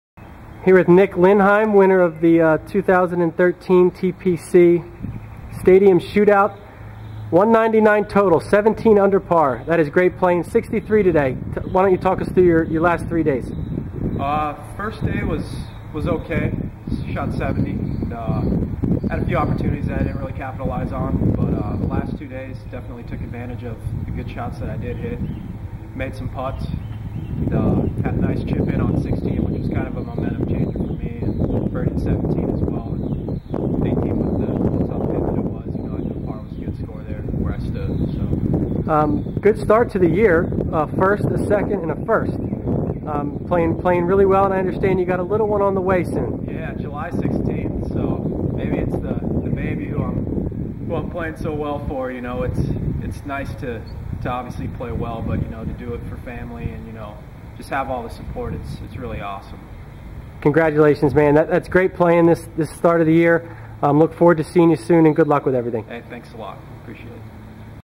Nick Lindheim 13' Summer Tour - Week 3
Nick Lindheim talks about his 2nd win this year on the West Florida Golf Tour and what its like getting ready to be a father for the first time.